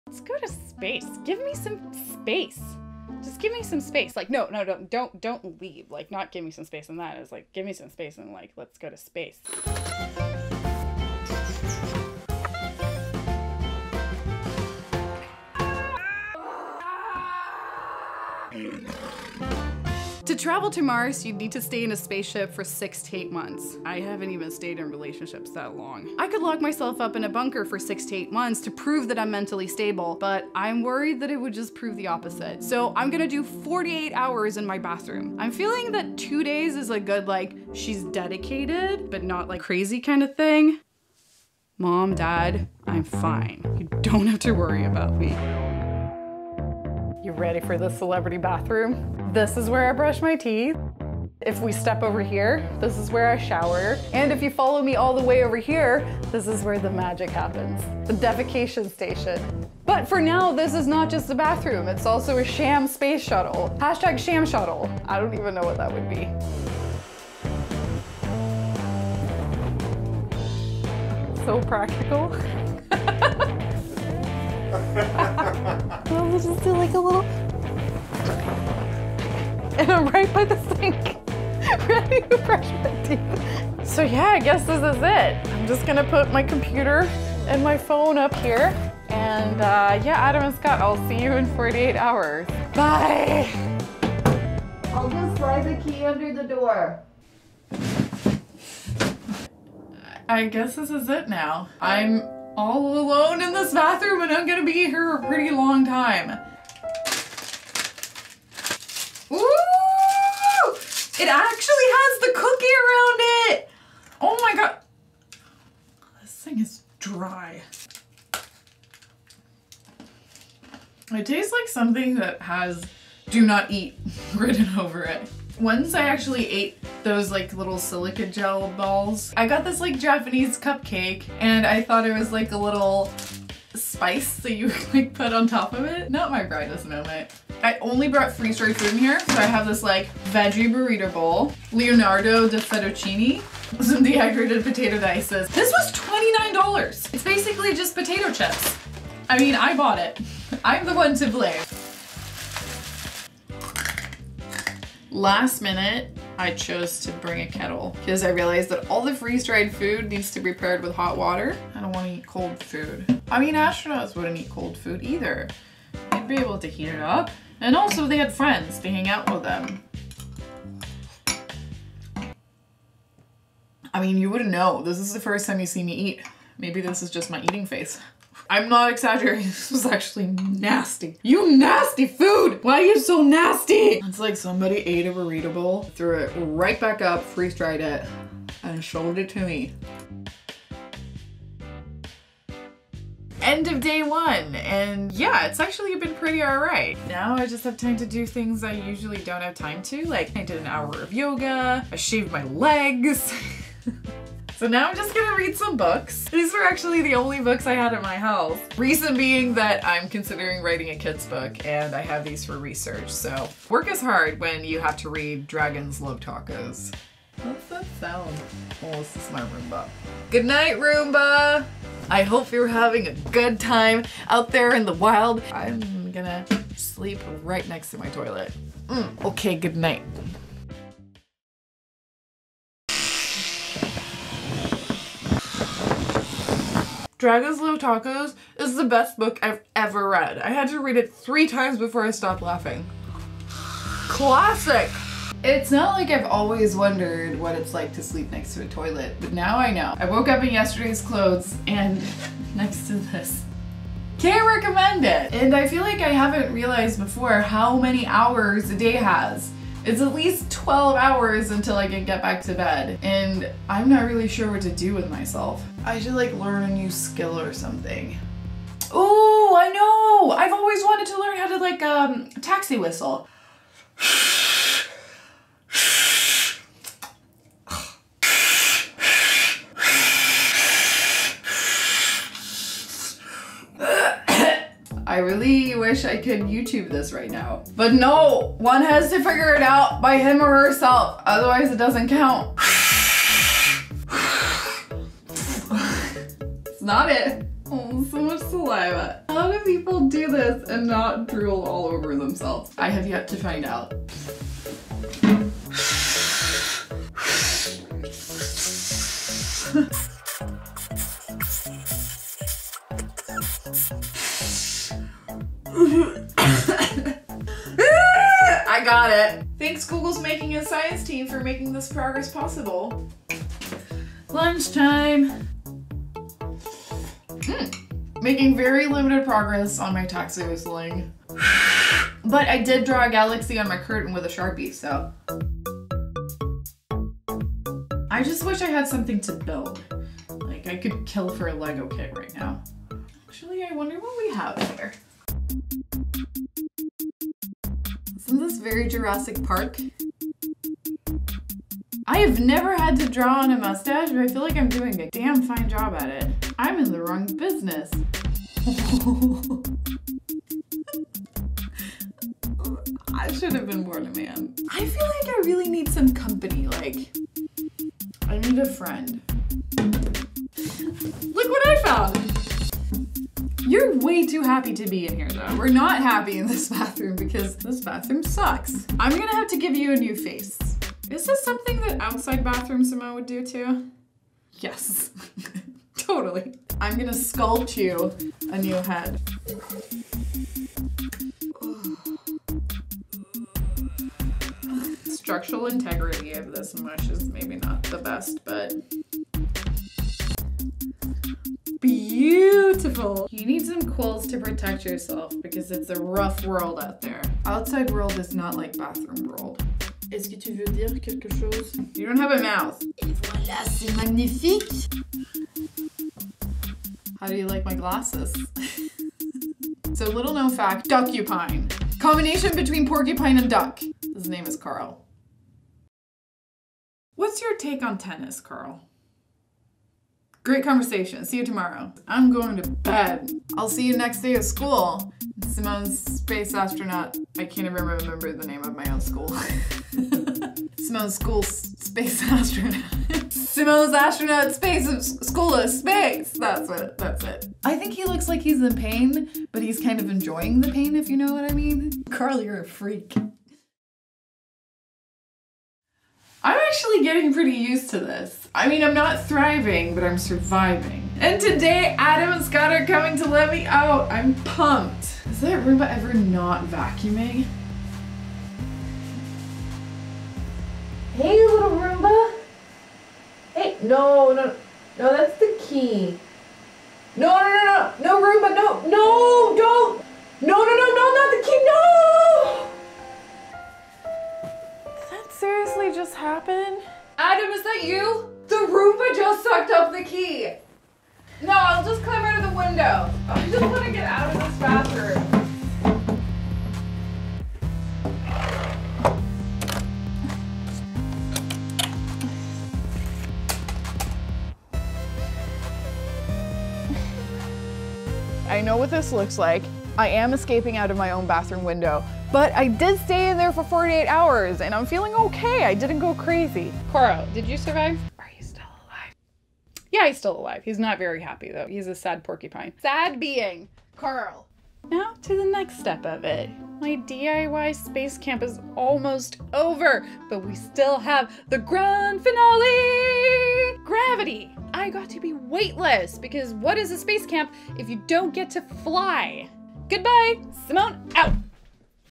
DIY Astronaut Training | 48hr Bathroom Lock in with Simone Giertz
This DIY astronaut training experiment that Simone Giertz devised involves spending 48hrs locked in her bathroom to experience that same isolation astronauts do in space. Also check out her zero gravity training!

This GoldieBlox video featuring Simone Giertz is about DIY astronaut training for space travel. She locked herself in her bathroom for 48 hours to get herself ready.